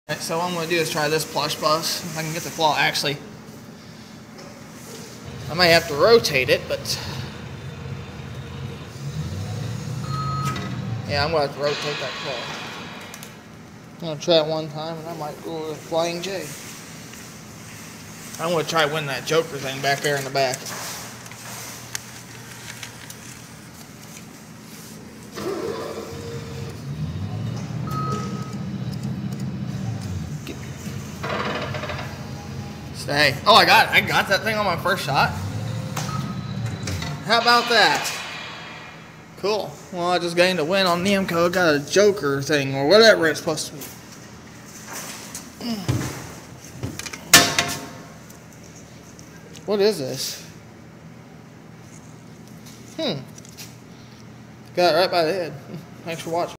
PLUSH BUS CLAW @WALMART EASY D.C. HEREO WIN
THIS WAS FILMED A FEW DAYS AGO AT MY LOCAL WALMART HERE I AM PLAYING THE PLUSH BUS CLAW MACHINE THIS ONE IS 1 DOLLAR PER PLAY  I HOPE YOU ENJOY